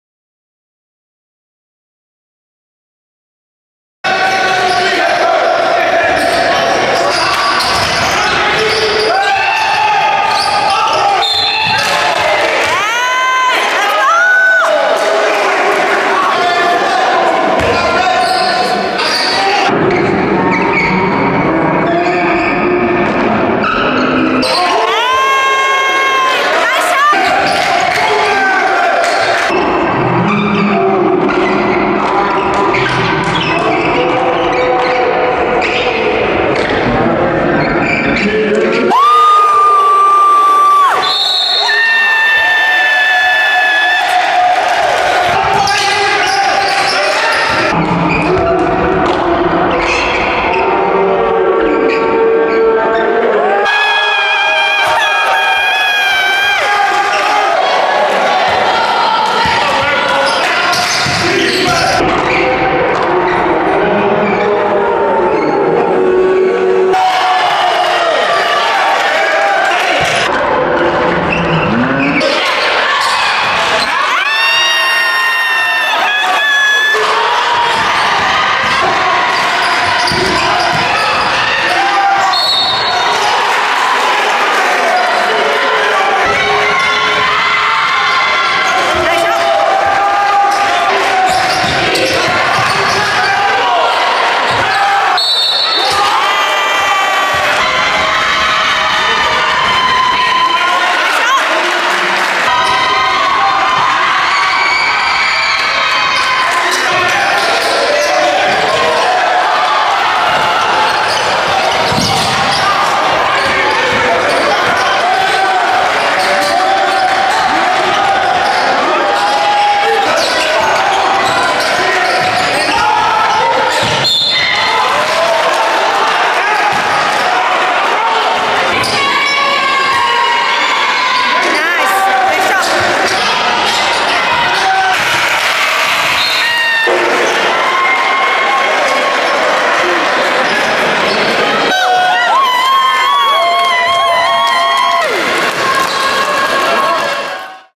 #2 Robert Phelps I PLAYOFF Highlights I Bedford Academy vs Franklin K Lane I 3/4/24
Athlete: Robert Phelps #2 (Junior)
League: PSAL, Division AAA, Brooklyn III
PSAL Official Playoff Game: 2nd Round
Game: Bedford Academy vs  Franklin K. Lane
Date: March 4, 2024
Location: Brooklyn, NY